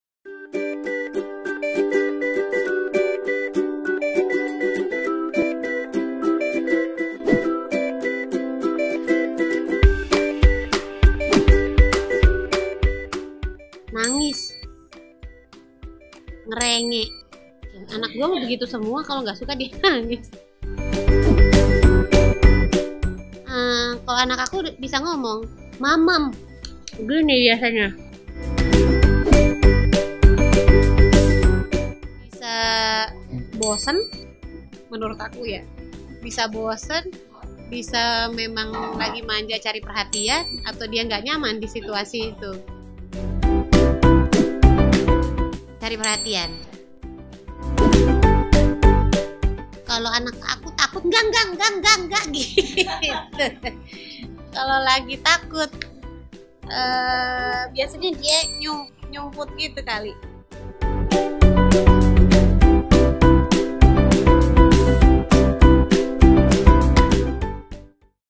Tantangan untuk Nycta Gina: Sudahkah Mengerti Bahasa Anak?
Kali ini Nycta Gina ditantang untuk menjawab pertanyaan dari Guesehat seputar anak pertamanya! Kira-kira Mums Nycta Gina bisa enggak ya?

Kamu juga bisa selalu update tentang informasi kesehatan, percintaan, bahkan fakta menarik lainnya dengan

Stay healthy!

Stay healthy!